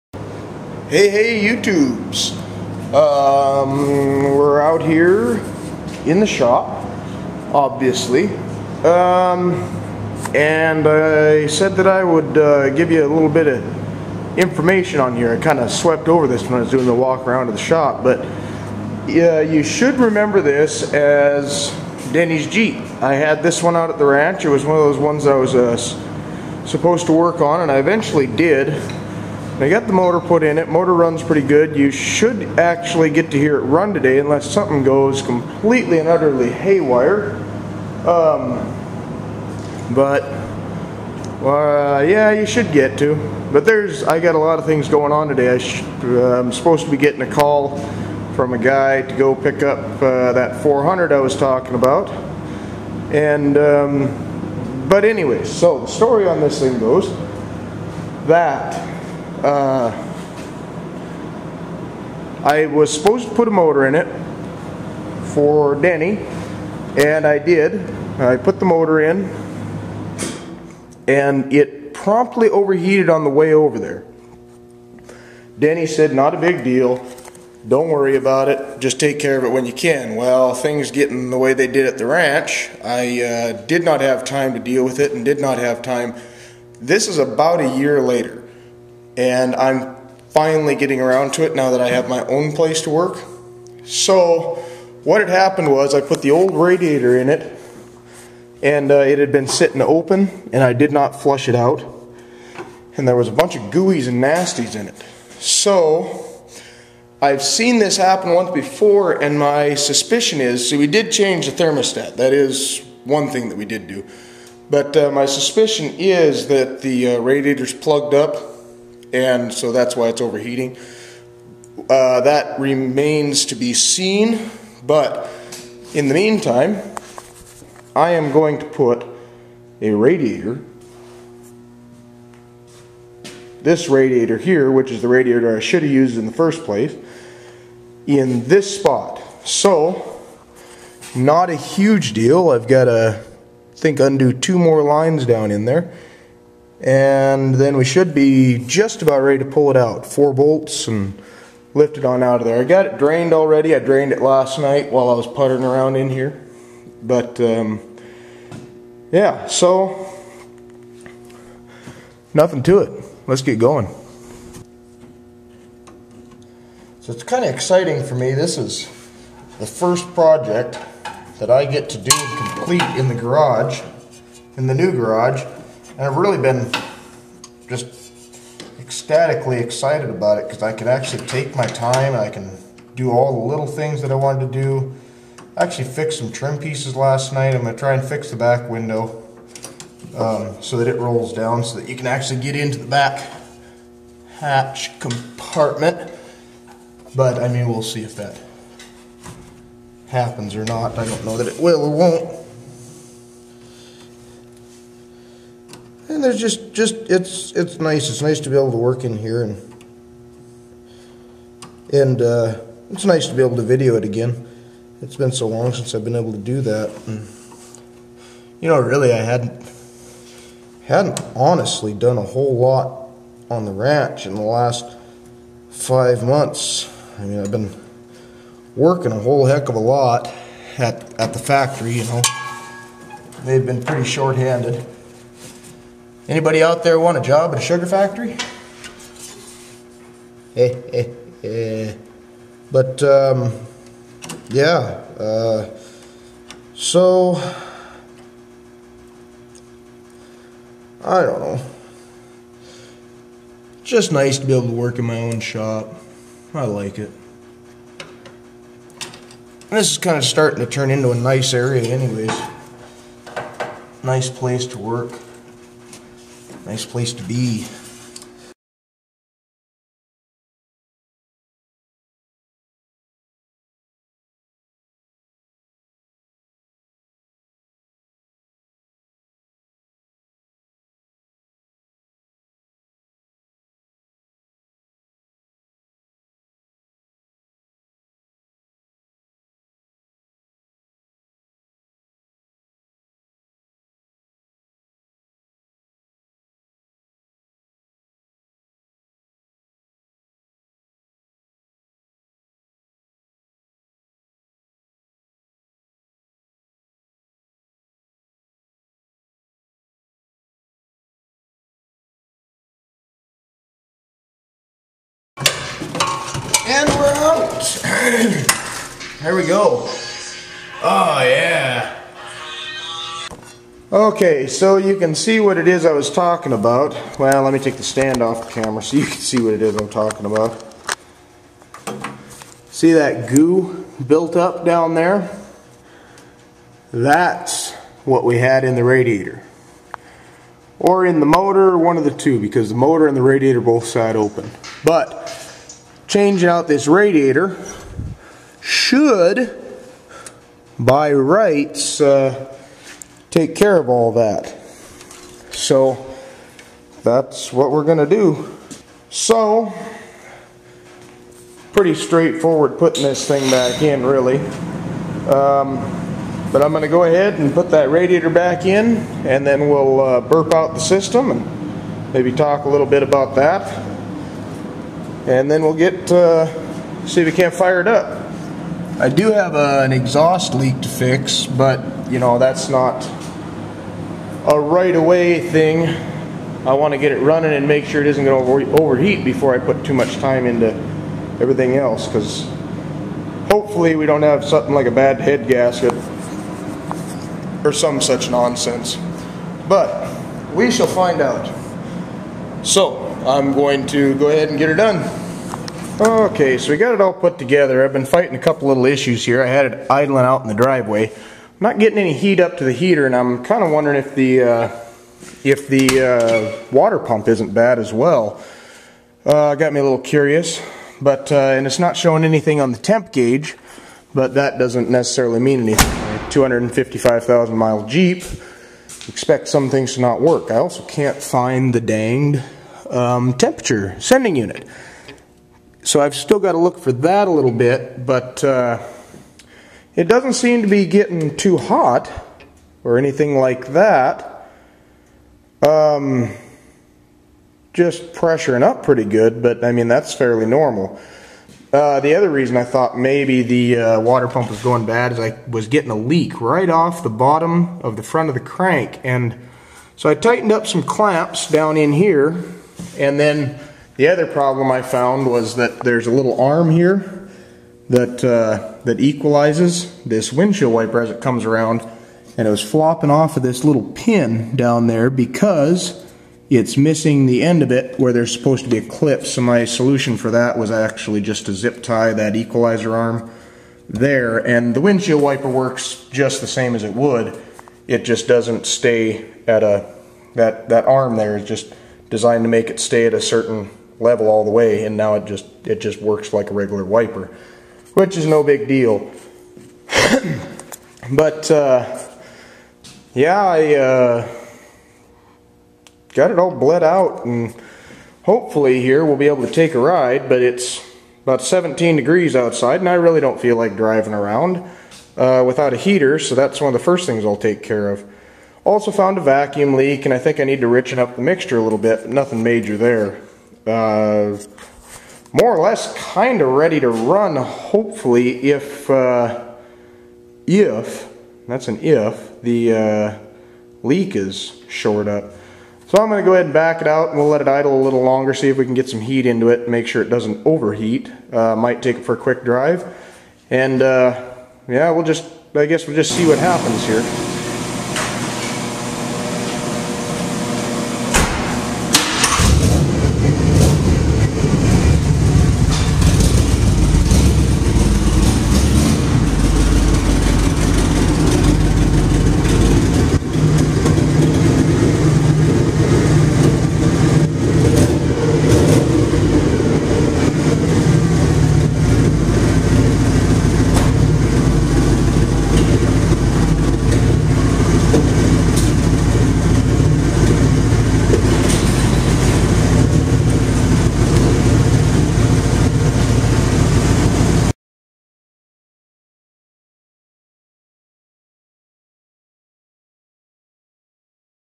Fixing Denny's Jeep (Again) Part
gotta be honest not my favorite video... I think I was actually having troubles with the camera after not having done mechanical videos in front of it for so long, I assure you the next will be better, got into this jeep pretty far so this will be a 3 or 4 part series